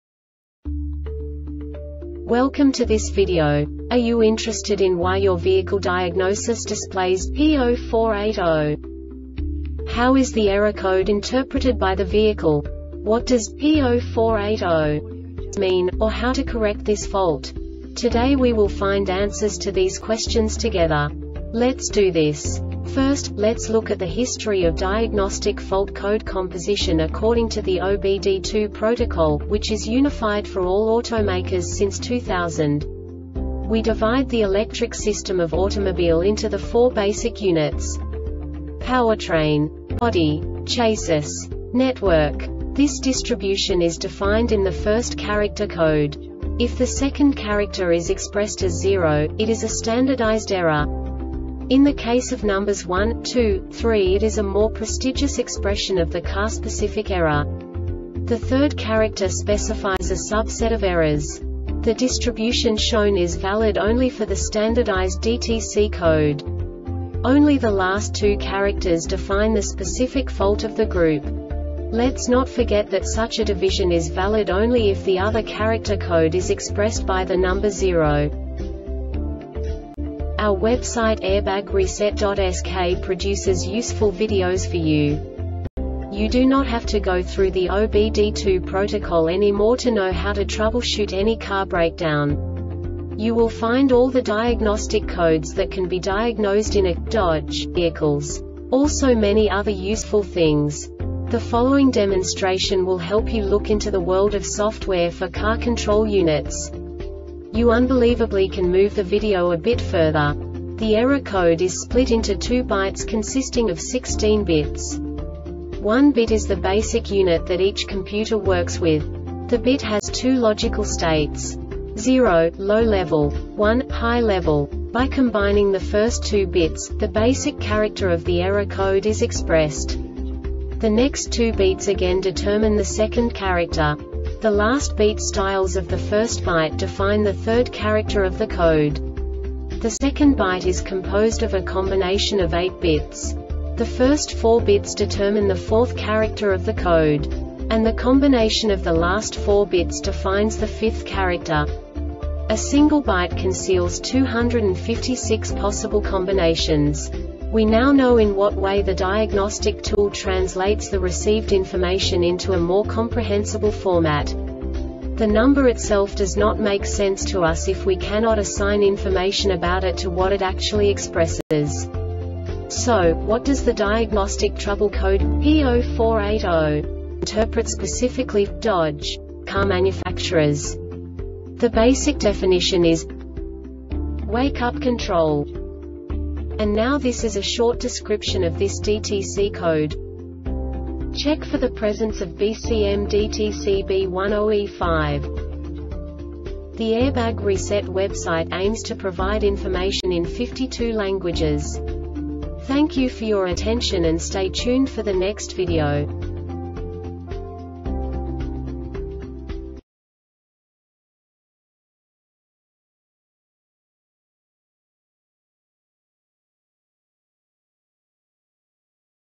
DTC Dodge P0480 Short Explanation
Contents:
0:21 Basic DTC analysis according to OBD2 protocol standard.
1:48 Insight into programming
2:58 A diagnostically understandable description of the Diagnostic Trouble Code
3:05 Basic error definition

Make:
Dodge
Code:
P0480
Definition:
Wakeup Control
Diagnosis: